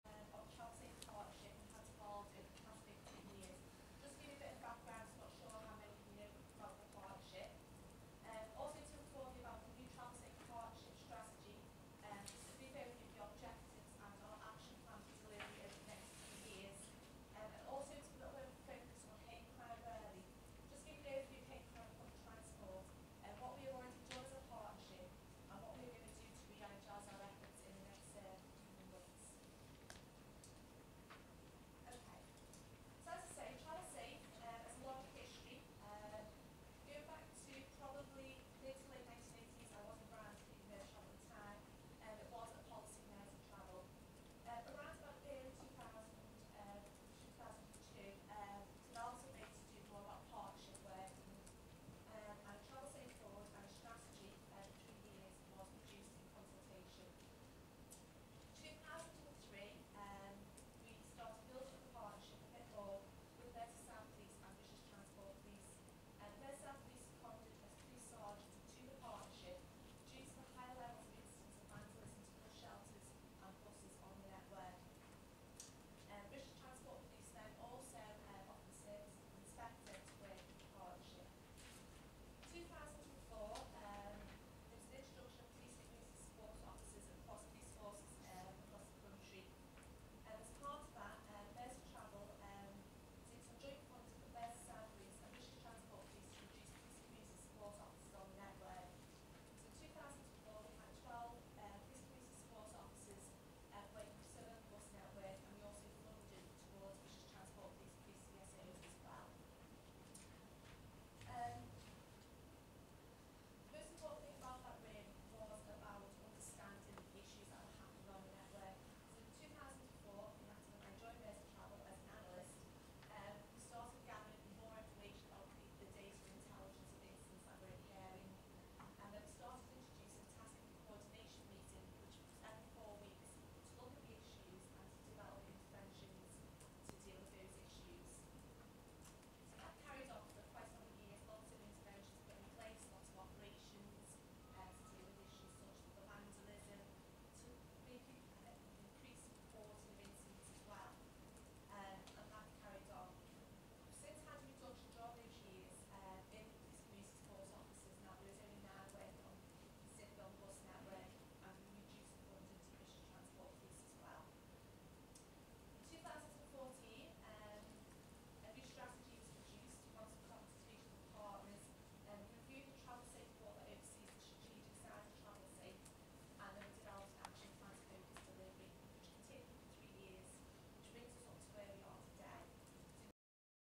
Transport Committee (Liverpool City Region Combined Authority) 5th April 2018 slides part 1 of 3
Authority Chamber, 1st Floor, No 1 Mann Island, Liverpool, L3 1BP

Present:
Cllr Les Rowlands
Cllr Liam Robinson
and others

The agenda and reports for this meeting can be found on Merseytravel's

7. TravelSafe Presentation (Nicola Swanson) 0:01

To consider a presentation by Nicola Swanson on the TravelSafe Partnership, providing an overview of its strategic objectives including its plans to tackle Hate Crime.

Continues at Transport Committee (Liverpool City Region Combined Authority) 5th April 2018 slides part 2 of 3